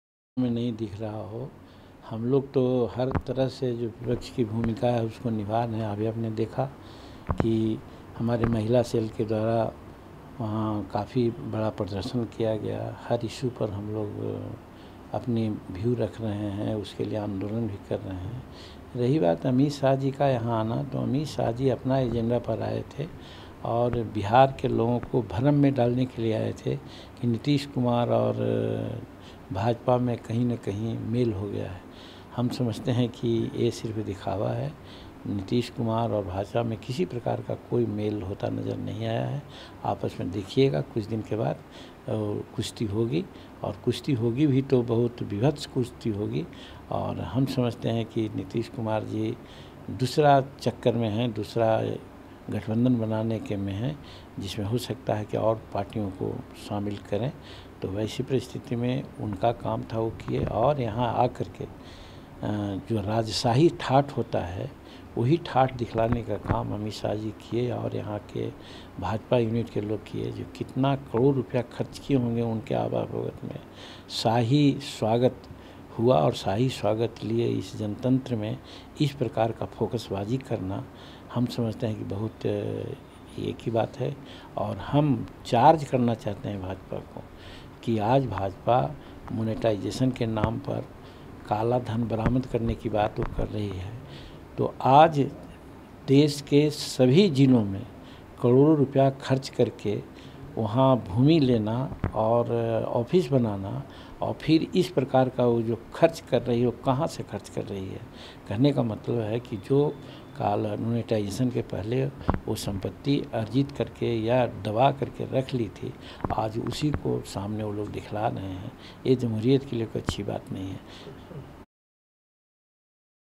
जीतन राम मांझी ने अमित साह को जम कर हमला बोला और
बिहार में बीजेपी बीजेपी अध्यक्ष को जम कर हमला बोला जीतन राम मांझी ने कहा जिस तरह से बिहार में उनको राजशाही जैसा पैसा खर्च किया है